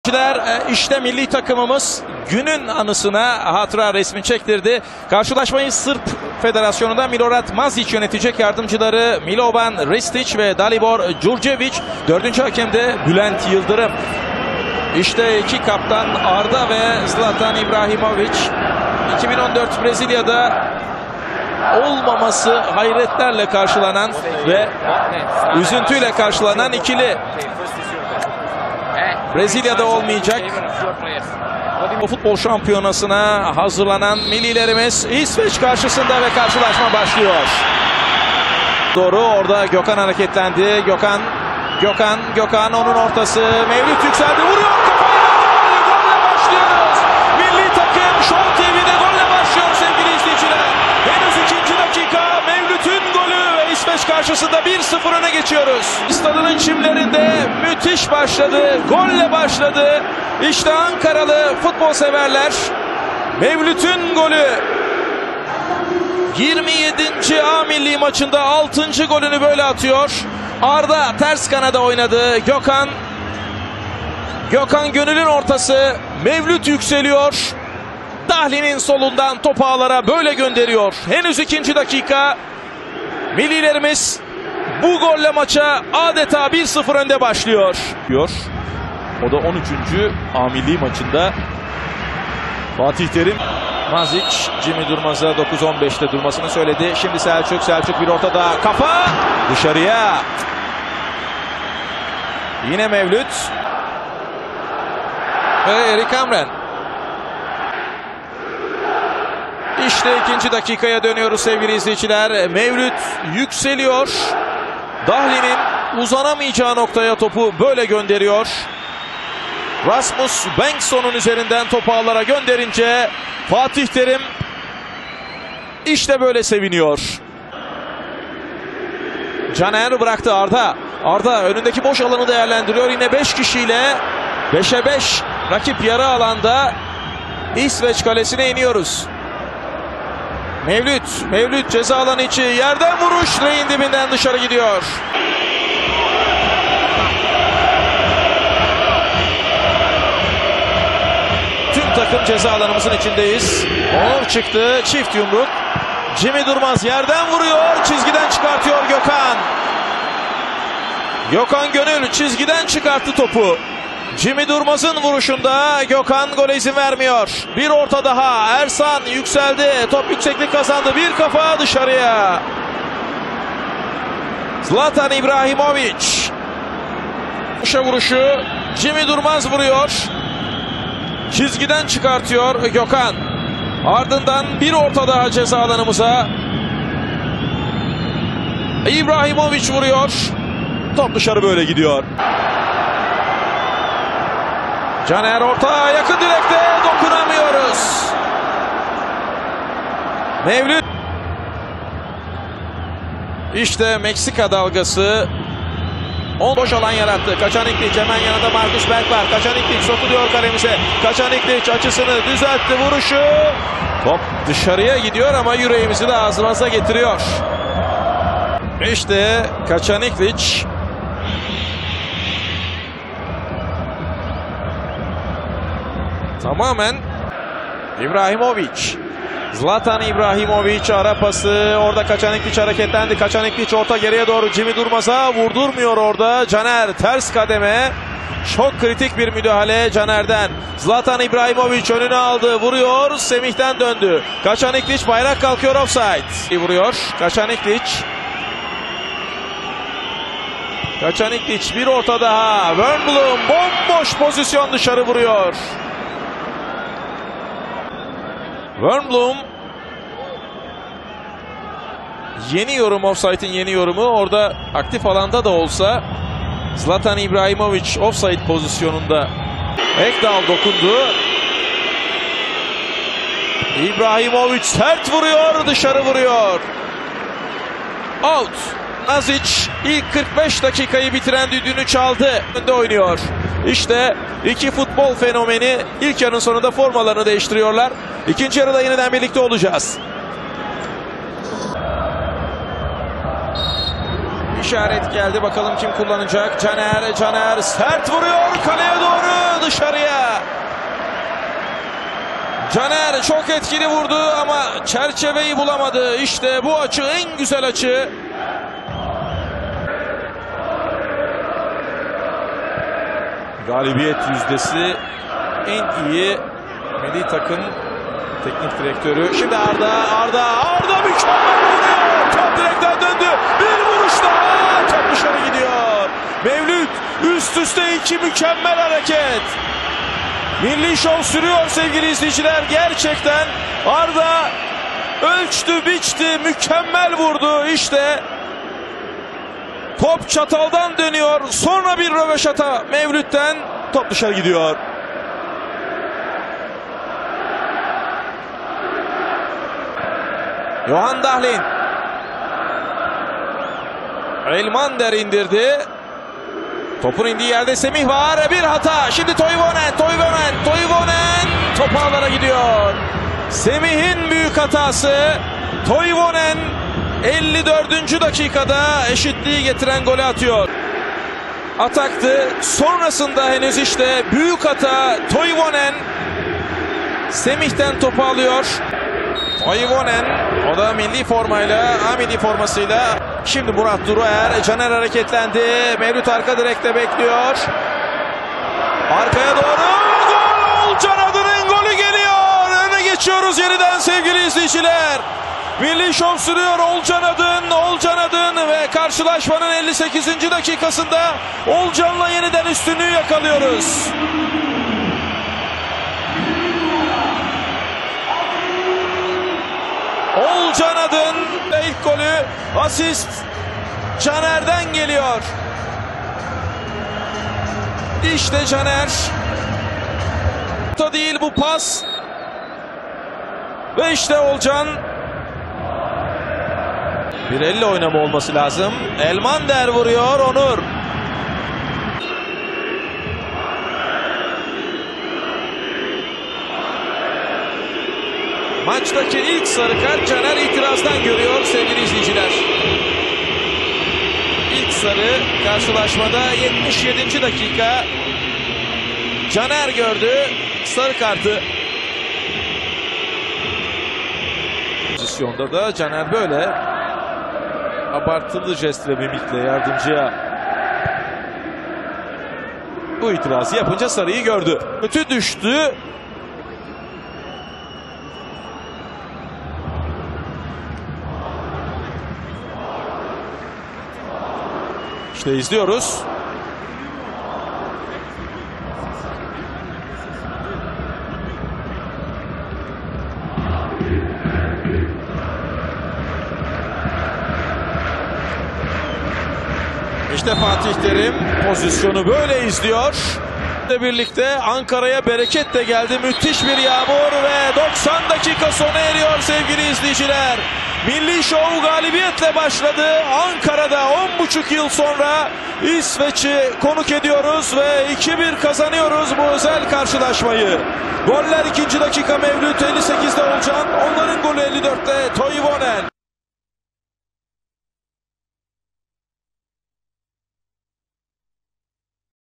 TÜRKİYE 2 - 1 İSVEÇ | 2014 - Geniş Özet
2014 Hazırlık Maçı - Ankara 19 Mayıs

Türkiye: Onur Kıvrak, Semih Kaya, Ersan Adem Gülüm, Caner Erkin, Alper Potuk (Dk. 46 Murat Duruer), Gökhan Gönül, Selçuk İnan (Dk. 67 Olcay Şahan), Nuri Şahin (Dk.91 Tarık Çamdal), Arda Turan (Dk. 90 Uğur Demirok), Burak Yılmaz (Dk. 46 Olcan Adın), Mevlüt Erdinç (Dk. 84 Umut Bulut)

İsveç: Johan Dahlin, Bengtsson, Antonsson (Dk. 46 Per Nilsson), Sebastian Larsson, Albin Ekdal, Kacaniklic, Zlatan Ibrahimovic (Dk. 46 Toivonen), Bengtsson (Dk. 46 Ekstrand), 16-Wernbloom (Dk. 84 15-Hiljemark), 19-Marcus Berg (Dk. 46 Johan Elmander), 21-Jimmy Durmaz (Dk. 67 Muamer Tankovic).

Goller: Dk. 2 Mevlüt Erdinç, Dk. 57 Olcan Adın (Türkiye), Dk. 54 Ola Taivonen (İsveç)